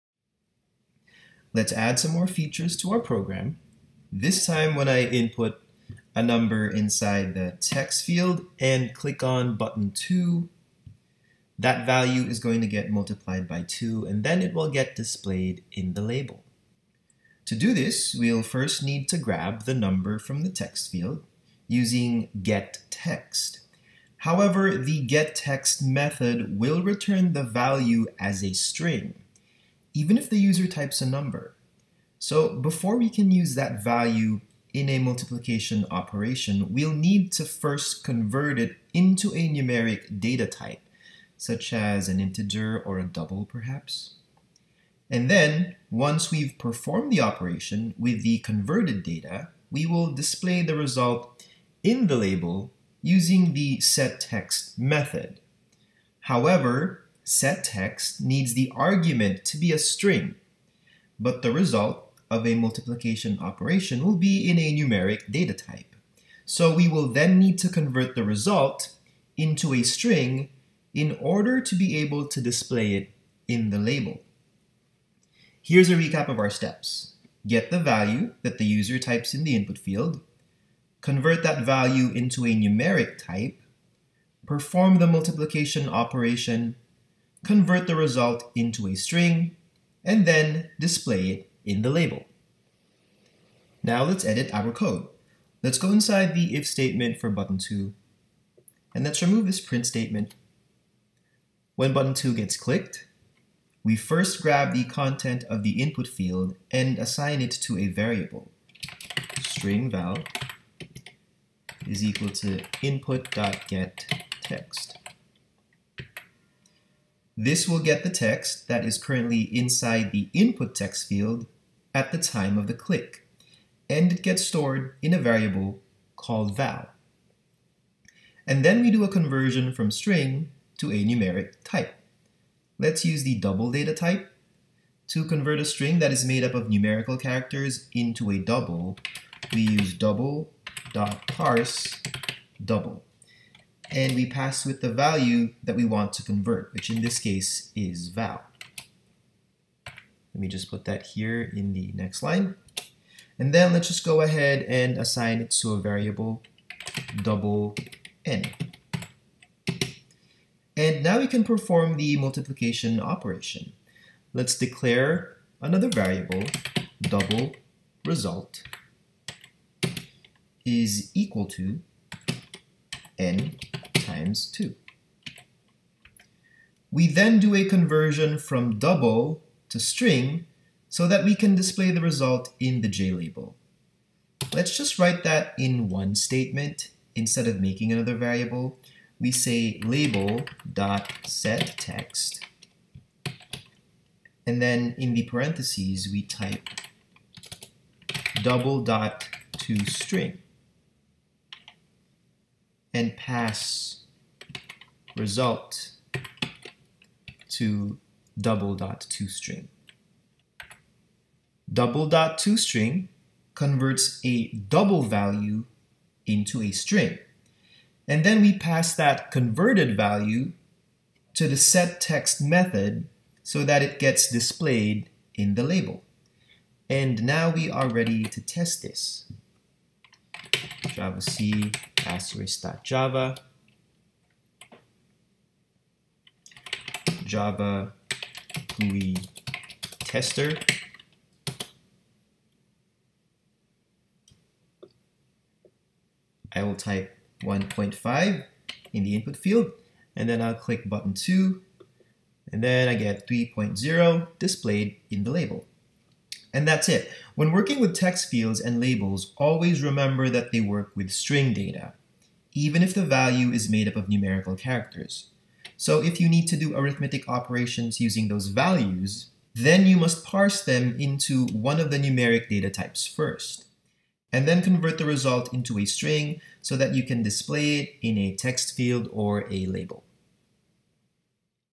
Part 10 - Converting Strings to Numbers and Numbers to Strings | Java GUI Tutorial
In this video, we'll convert values from Strings to numerics and vice versa ( e.g. Double.parseDouble() and Double.toString() ), so that we can perform arithmetic operations on values taken from text fields (JTextField), and then display the result in labels (JLabel).